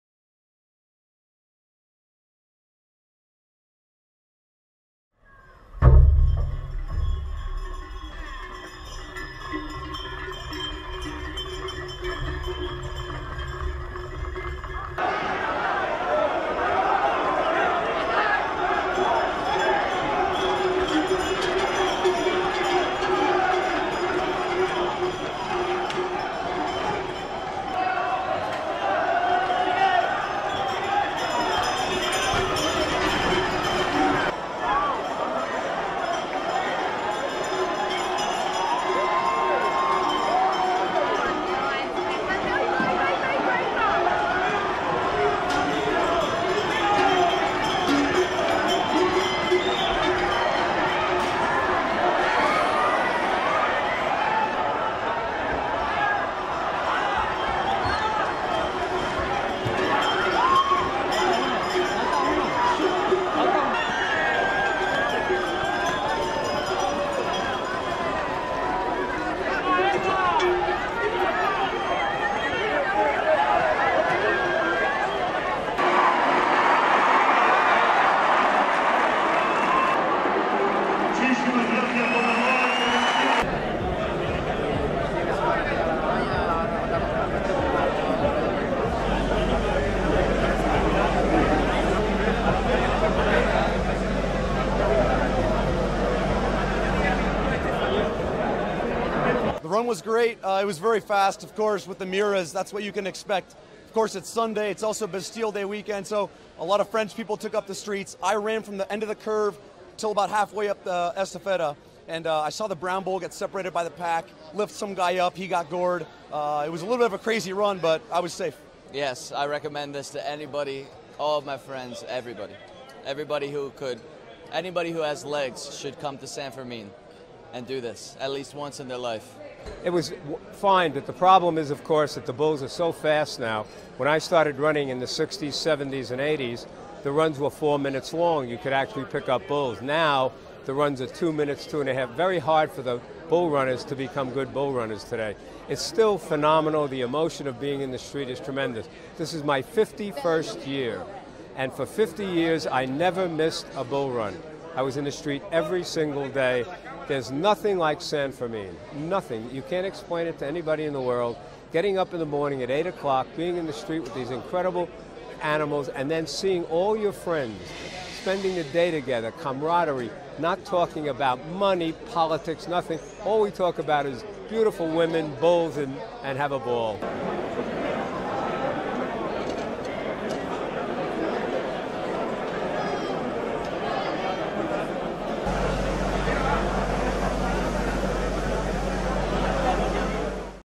Eight hospitalised on last day of Pamplona bull-run
Rabanero the bull broke away from its pack of six and gored three tourists on the last day of the San Fermin festival in Pamplona. One was gored in the leg and slammed on to the cobbled streets and two others were gored in the arm and armpit. They were all rushed to hospital but their wounds are reportedly not life threatening. Rabanero the bull broke away from its pack of six and gored three tourists during the San Fermin festival in Pamplona. In total eight participants were injured on the festival's final day today. Some 16 people have died in the bull runs since records began in 1910. The last death was in 2009.